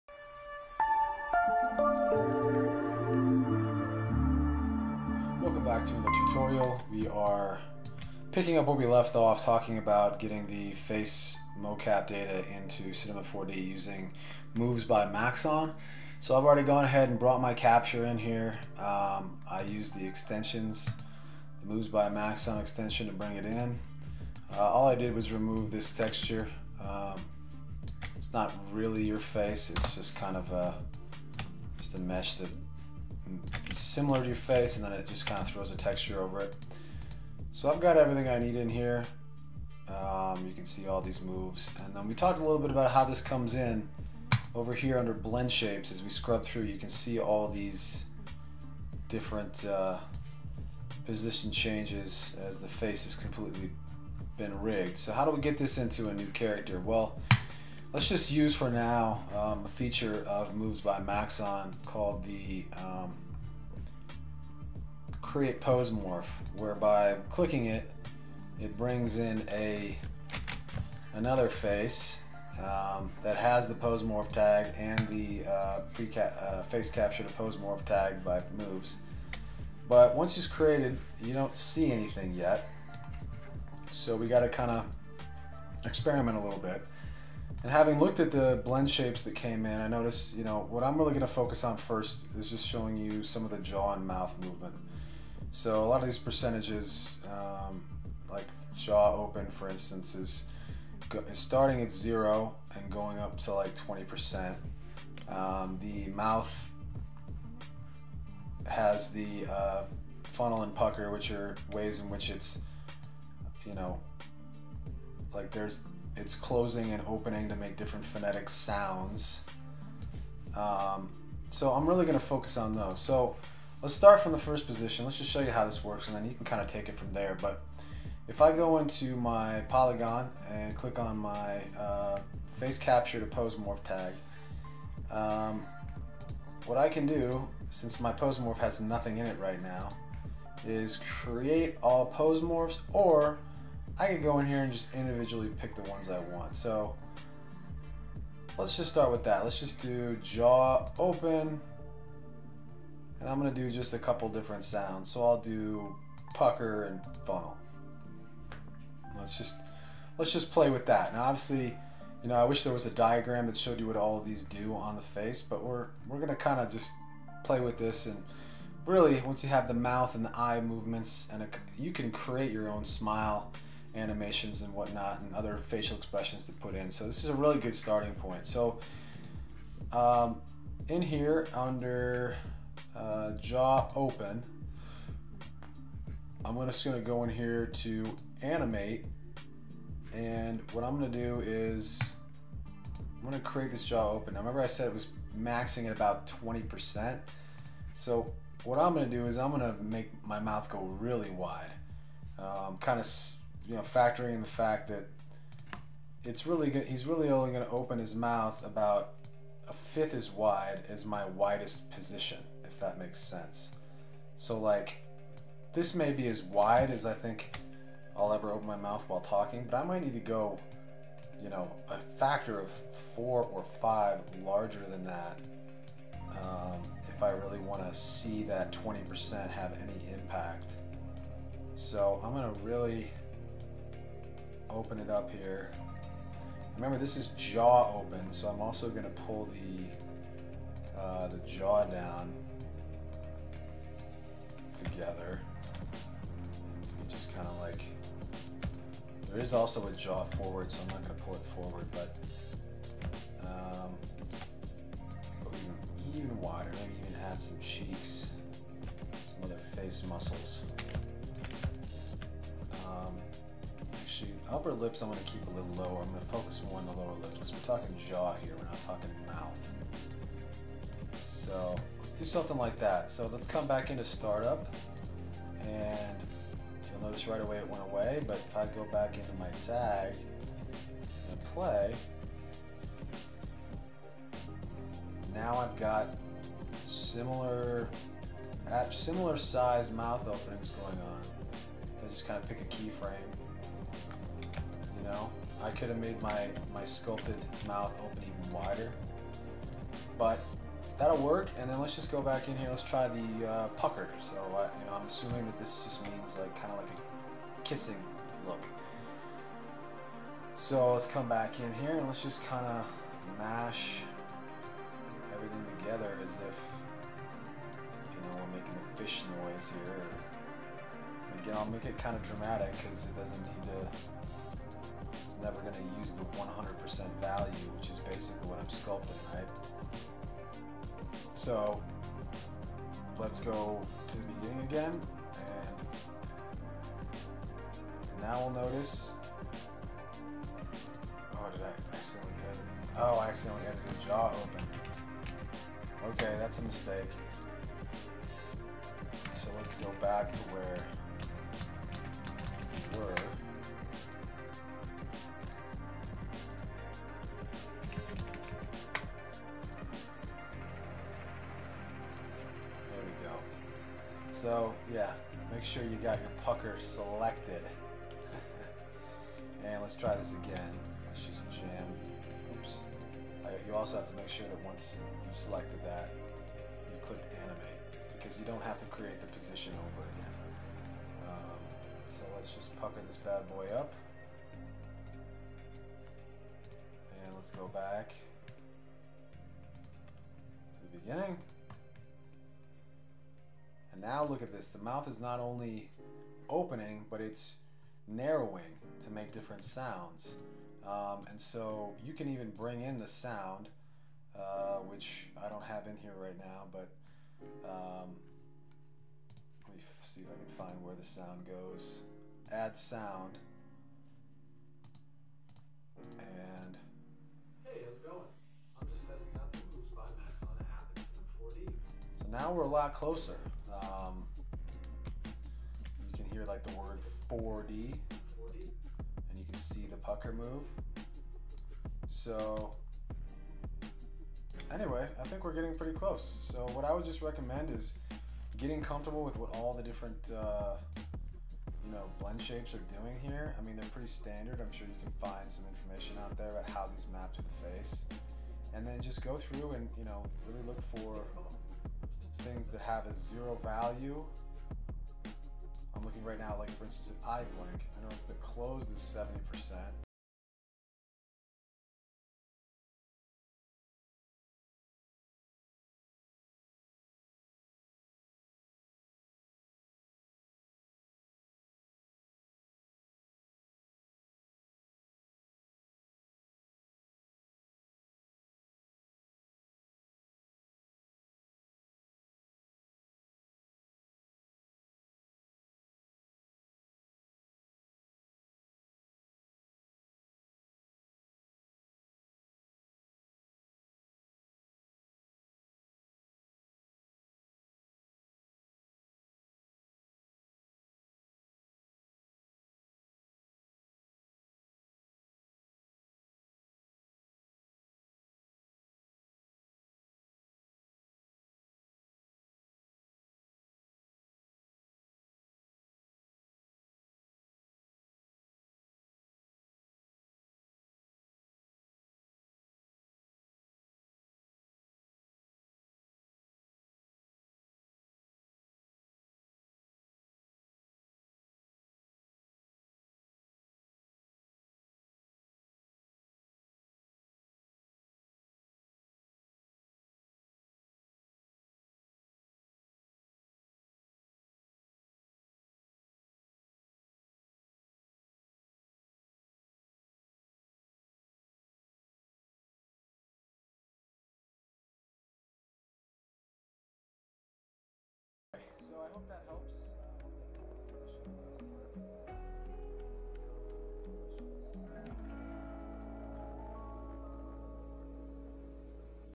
Part 2: Auto Rigging with facial capture data using Moves By Maxon in Cinema 4D
This tutorial is about auto rigging a face using pose morphs brought in from the moves by maxon app into Cinema 4D, using their facial capture system.